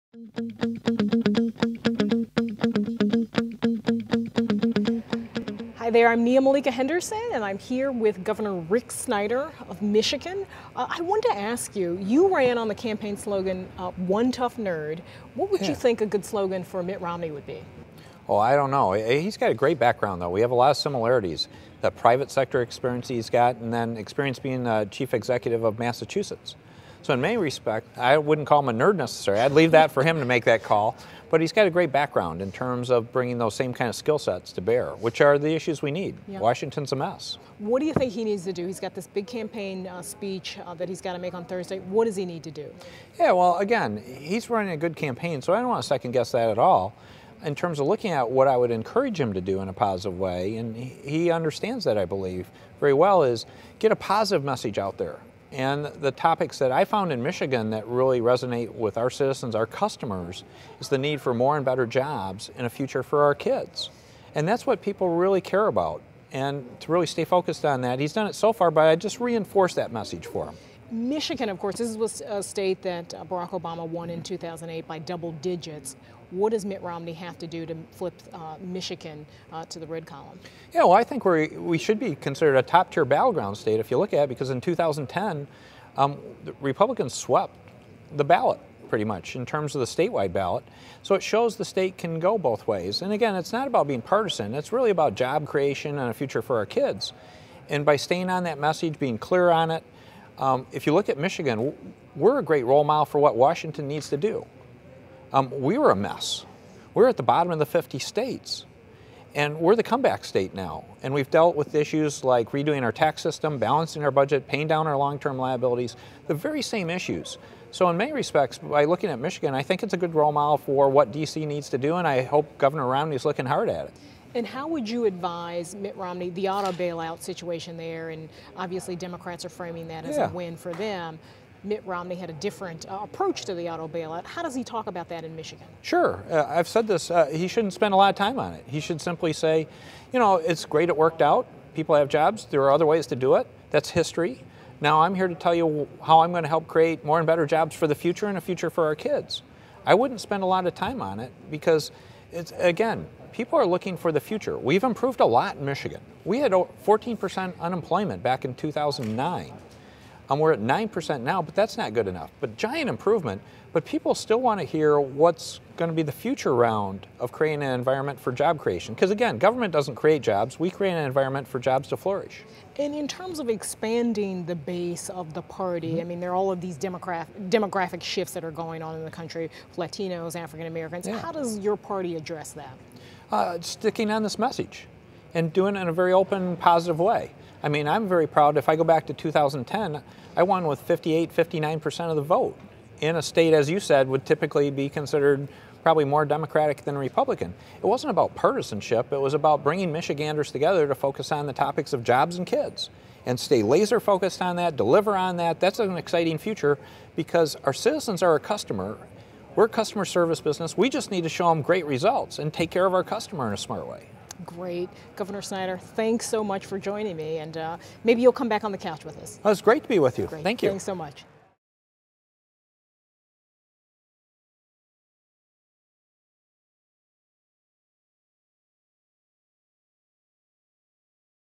Gov. Rick Snyder turning Michigan red
Gov. Rick Snyder (R-Mich.) speaks with The Post's Nia-Malika Henderson about how Mitt Romney can win Michigan voters.